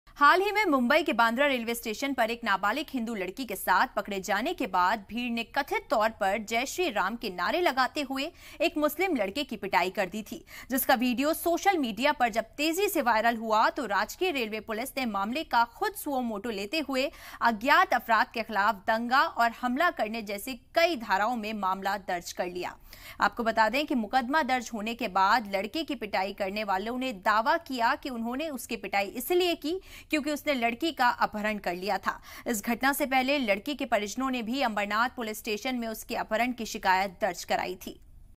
Maharashtra News: मुस्लिम शख्स की पिटाई मामले में पुलिस ने किया केस दर्ज
Maharashtra News: मुस्लिम शख्स की पिटाई मामले में पुलिस ने किया केस दर्ज, जानें पूरा मामला
Maharashtra News: महाराष्ट्र के बांद्रा रेलवे स्टेशन पर एक नाबालिग हिंदू लड़की के साथ पकड़े जाने के बाद भीड़ ने कथित तौर पर 'जय श्री राम' के नारे लगाते हुए एक मुस्लिम लड़के की पिटाई कह दी है. स्थानीय पुलिस ने यह जानकारी दी है. पुलिस के मुताबिक, यह घटना 21 जुलाई की है. लेकिन घटना का वीडियो मंगलवार को सोशल मीडिया पर वायरल हुआ है. राजकीय रेलवे पुलिस  ने बुधवार को मामले का स्वतः संज्ञान लिया है. मुंबई के बांद्रा रेलवे पुलिस स्टेशन ने आईपीसी की धारा 141, 142, 143, 146, 147, 149, और 323 के तहत अज्ञात हमलावरों के खिलाफ मुकदमा दर्ज की है.